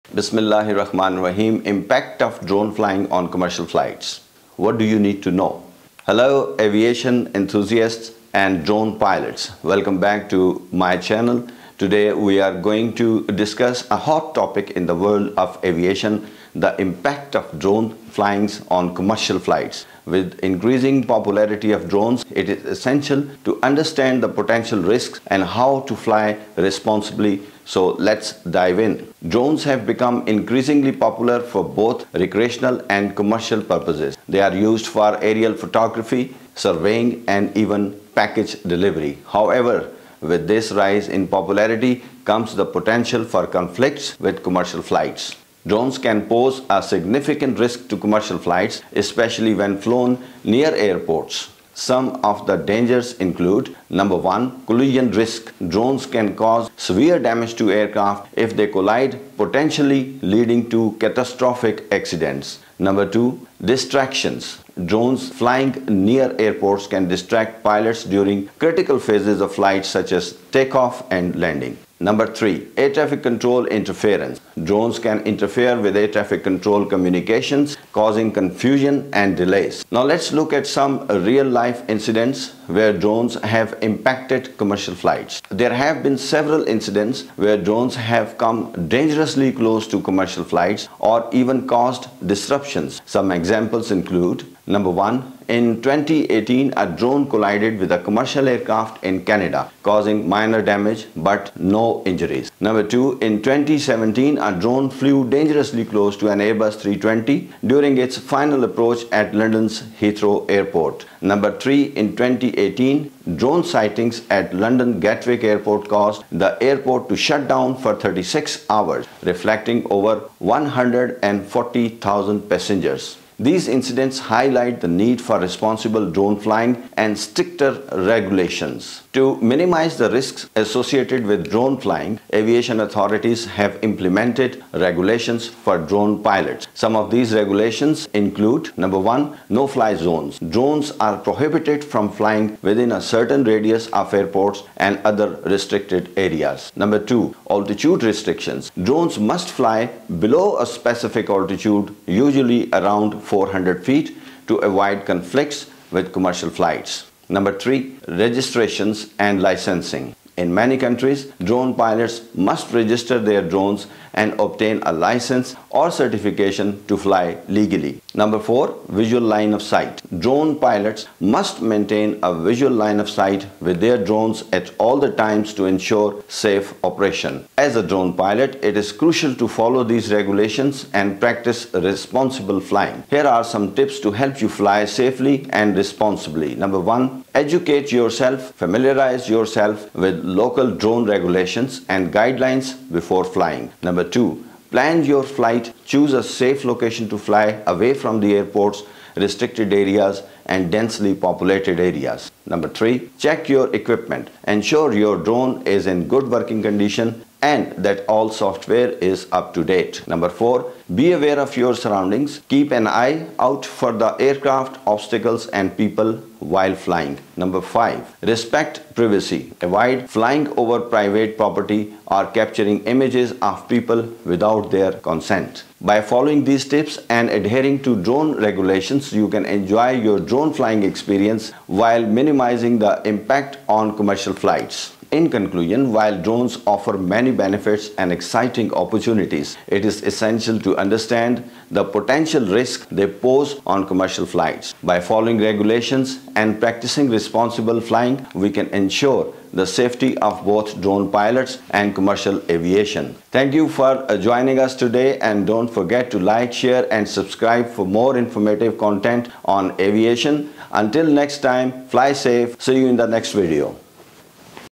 The Impact of Drone Flying on Commercial Flights: What You Need to Know
In today's video, we're going to talk about the impact of drone flying on commercial flights. We'll cover what you need to know about drone flying, how to safely fly a drone, and how to avoid drone collisions.

If you're worried about drone flying affecting commercial flights, then this video is for you! We'll discuss the dangers of drone flying, what you need to know about drone flying, and how to safely fly a drone. Learn what you need to know to keep yourself and other pilots safe while drone flying!